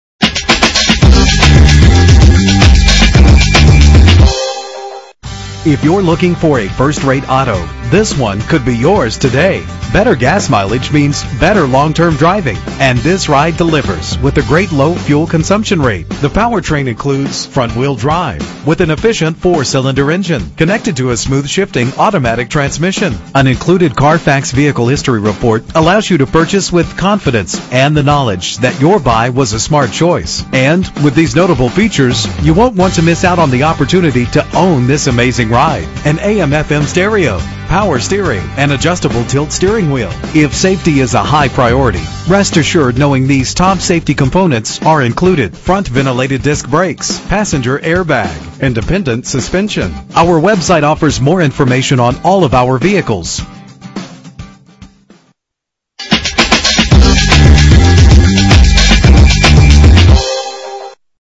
1999 Toyota Camry - Gladstone OR
Year: 1999
Make: Toyota
Model: Camry
Trim: LE
Engine: 2.2 liter inline 4 cylinder 16 valve
Transmission: 4-Speed Automatic
Color: Tan
Mileage: 118526
Address:
19400 SE McLoughlin Blvd